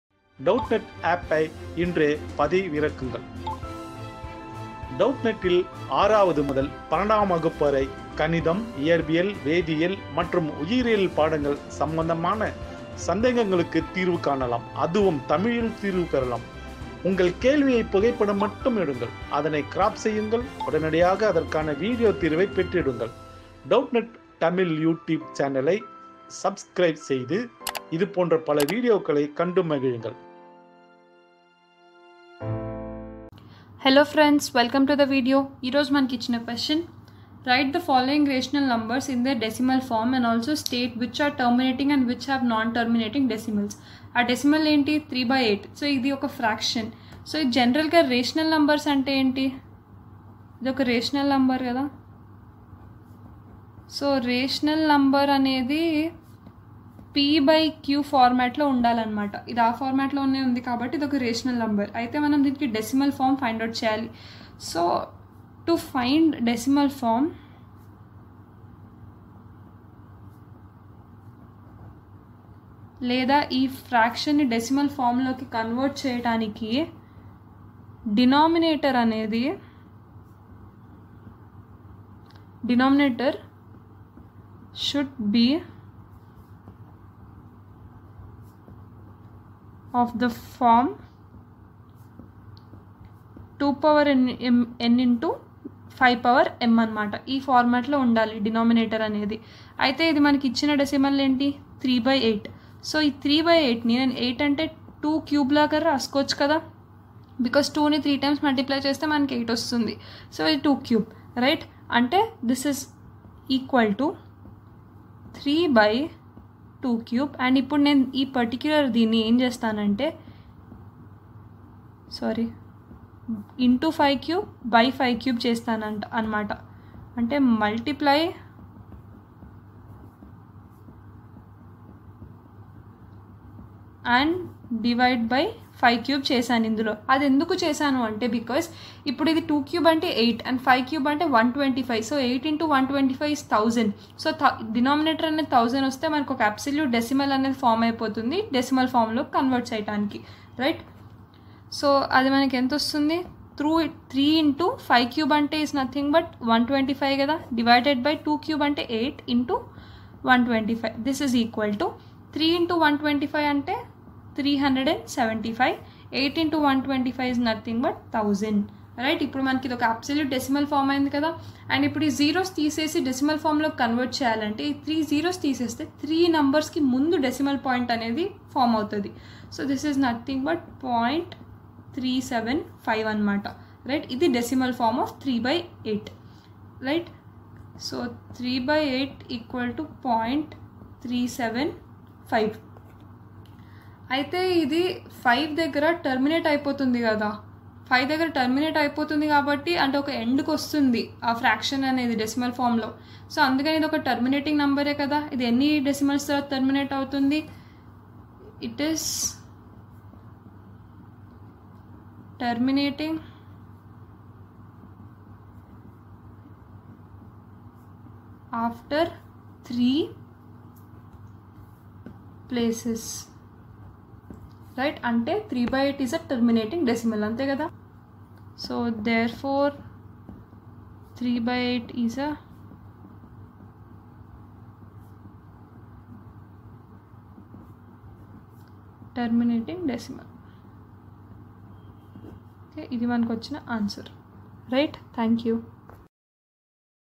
Write the following rational numbers in their decimal form and also | Class 10 MATH | Doubtnut
Write the following rational numbers in their decimal form and also state which are terminating and which have non-terminating repeating decimals. (i) `3/8`

Online Class में Admission लेने के लिए यहाँ click करें :
👉 Class 10 | CBSE Boards | English Medium :

👉 Have Any Query? Ask Us.